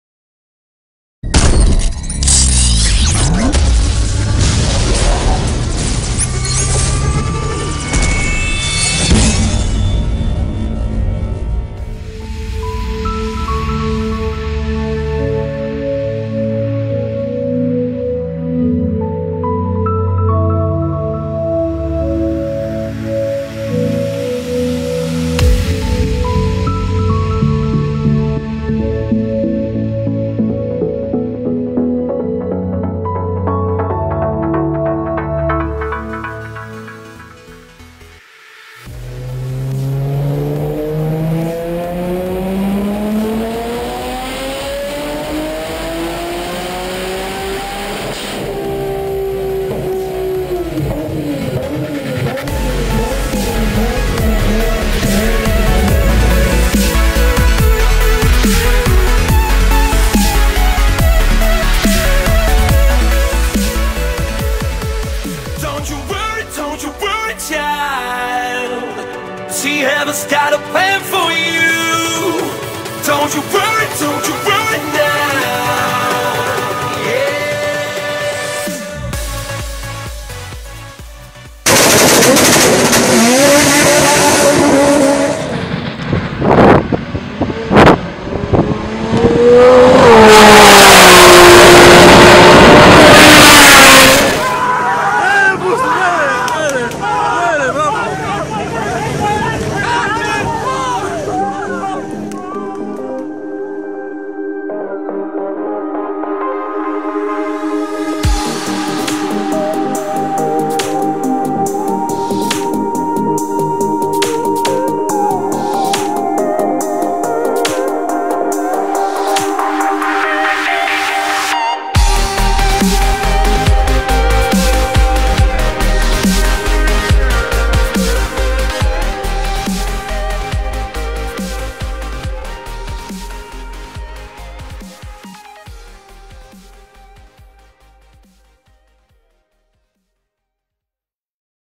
Exelixis GT-R 1500R 8.9seconds
GT-R EXL1500R at Drag Day Elliniko. The car was driven on its own wheels from Romania to Greece and back to Romania, 2500 kilometers! 8,983 seconds on its first run at a drag strip